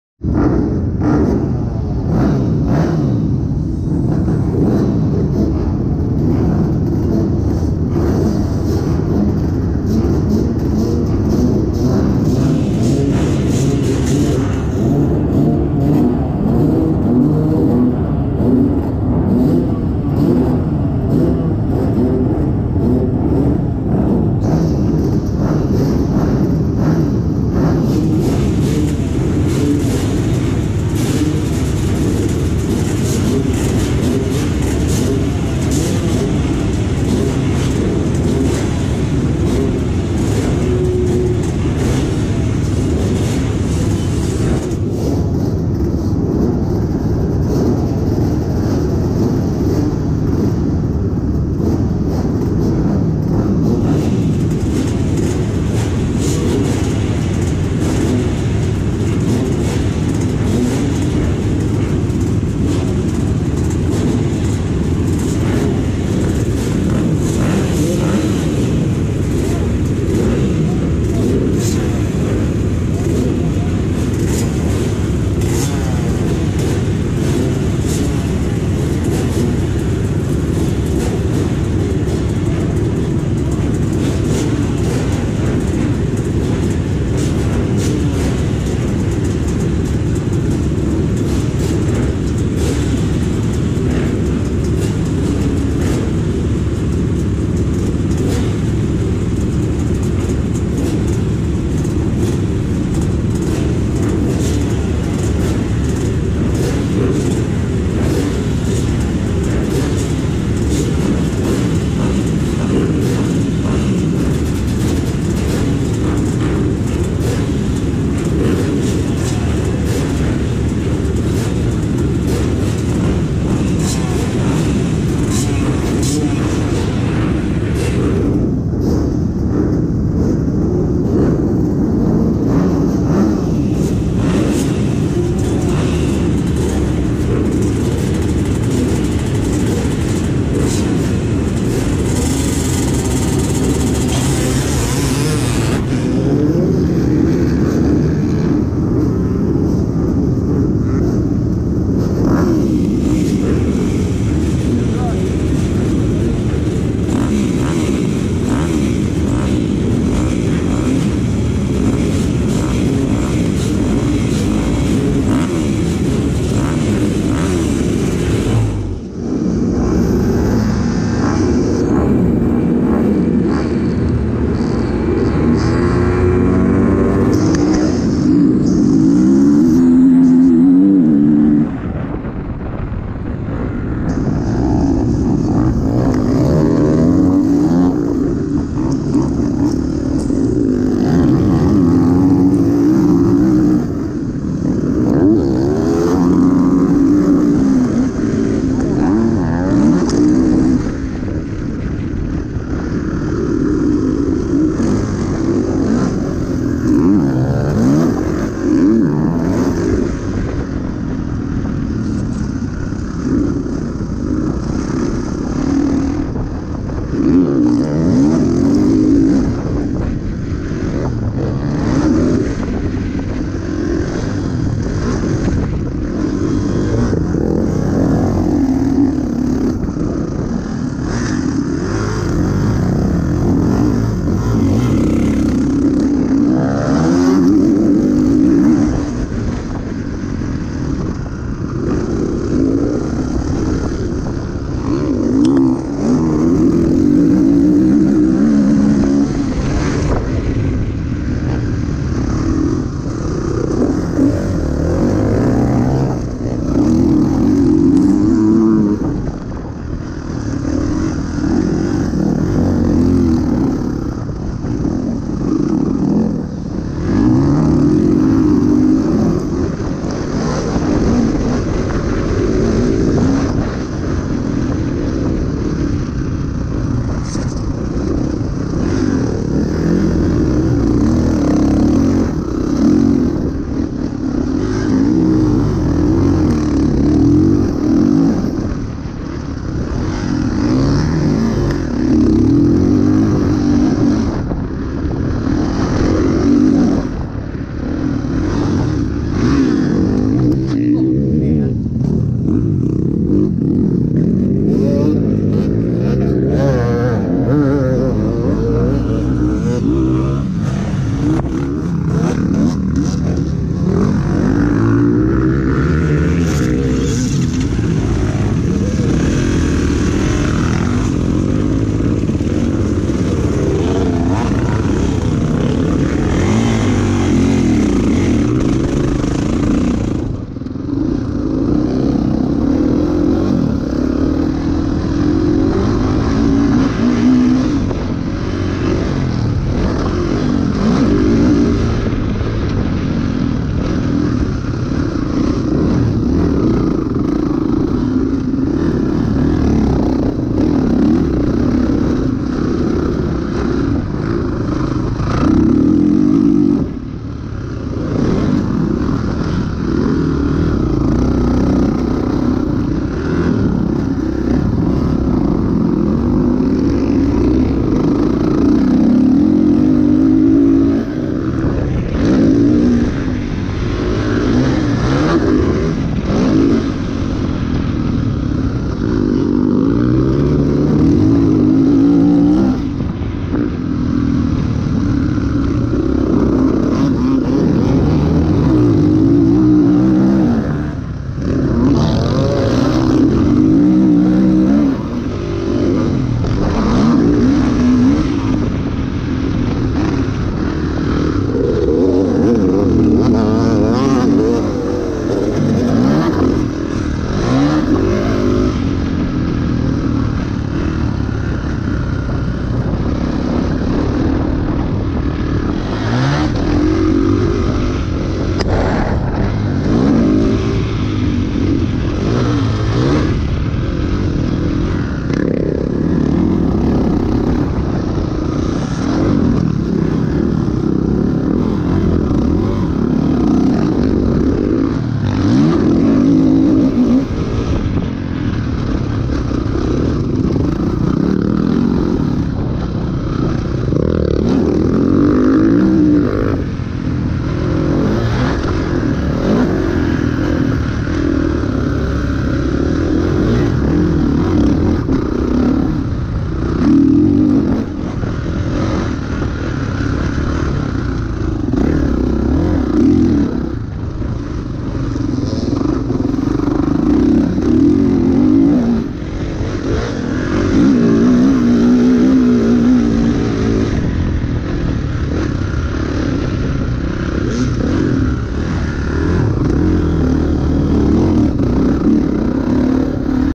Dutch Sport Park muskrat run vetsport moto1 eli9494 10/10/21
got a good start, was using that line all day, far outside and banking of a soft berm, anyway a new rut started developing out there, and I hit it by accident and crashed.